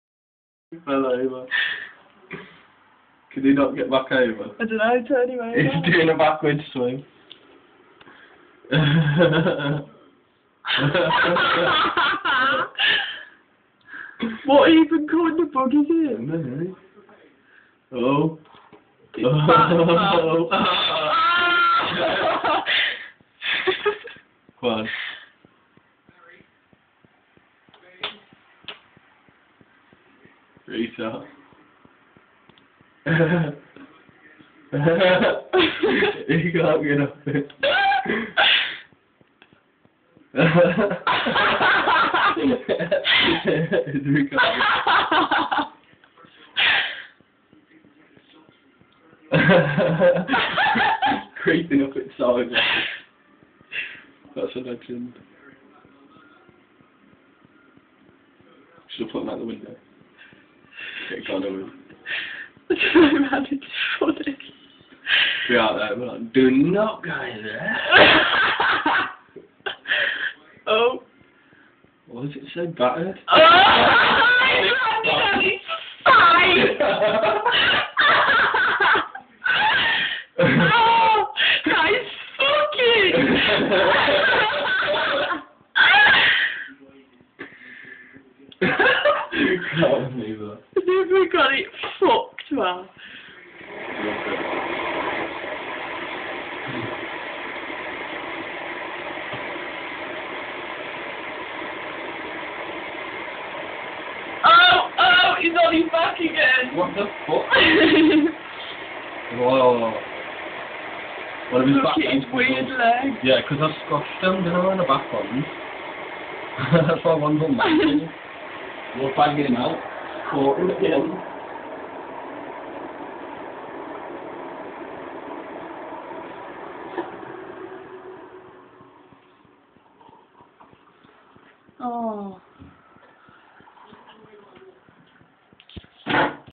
Stoned Bug :)
Me & my mate Jayden found a bug on the curtain in his lounge, so naturally we decided to put it in our bong & suck a bong with him swimming around in the bottom! After we did, we tipped him out. Here is the result, enjoy! :D